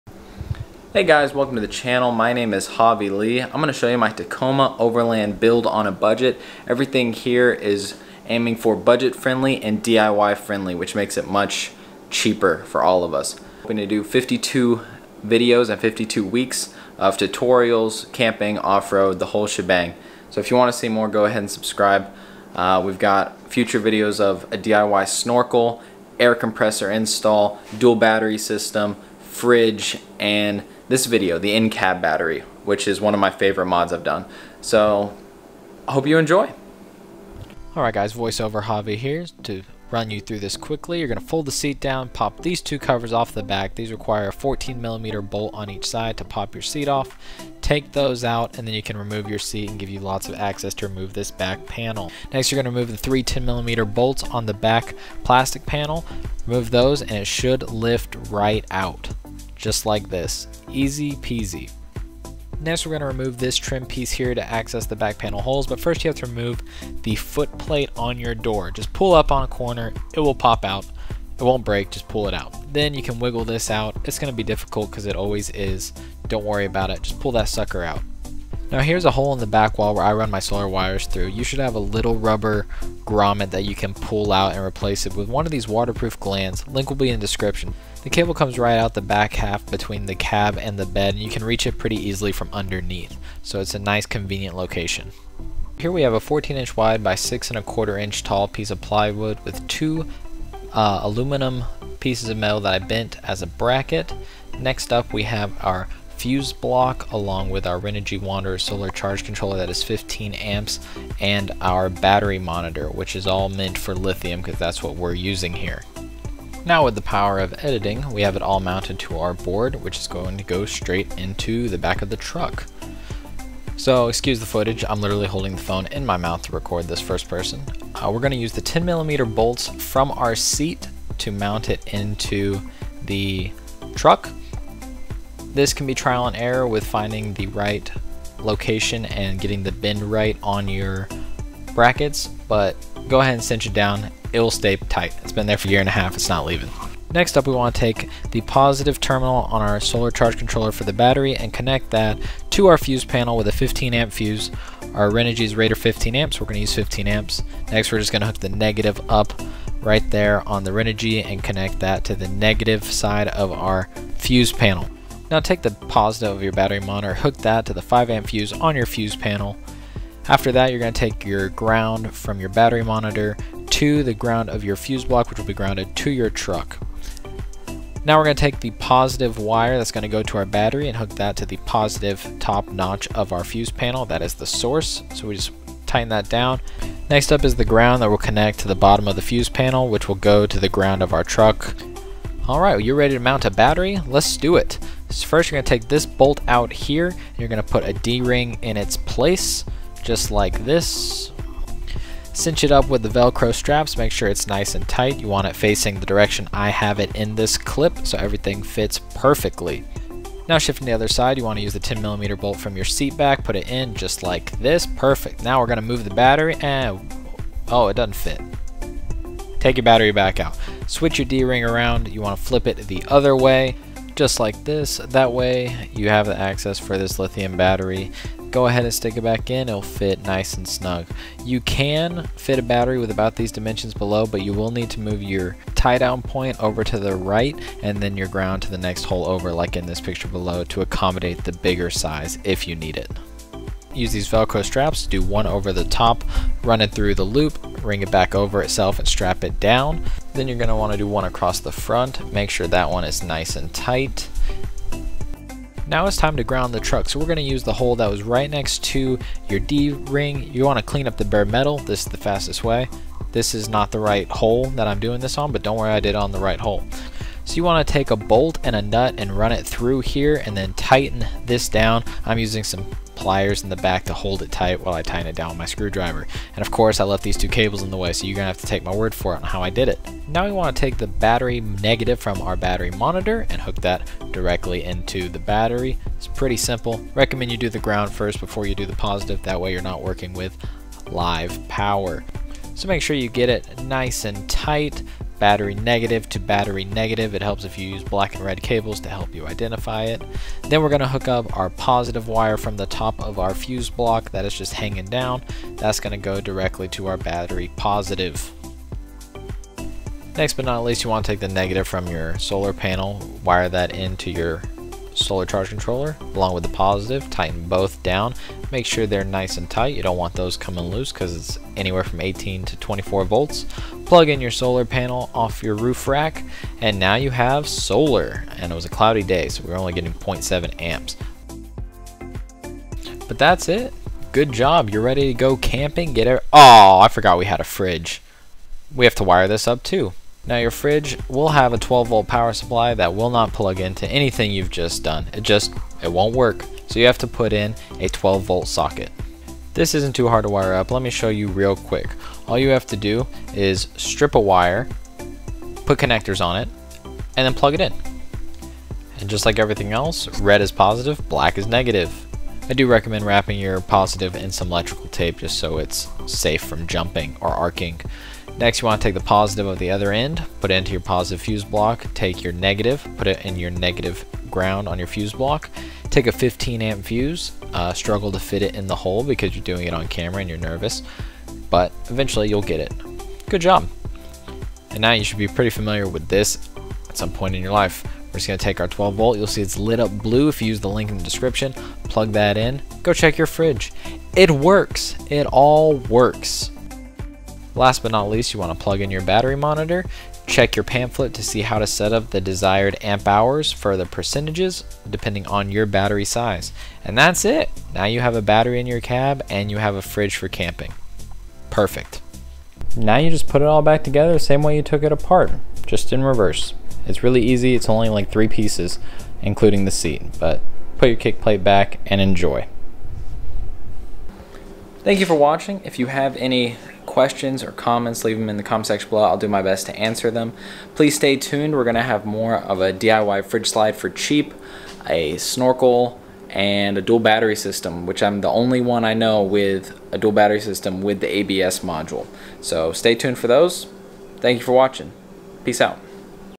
Hidden Battery for a Fridge Install - Tacoma Overland Build on a Budget
Today I'm going to show you one of the BEST locations to mount a lithium battery in your cab for a fridge, lighting, and accessories in your Tacoma!  This is a perfect spot to mount everything and hook it up to solar from your roof rack.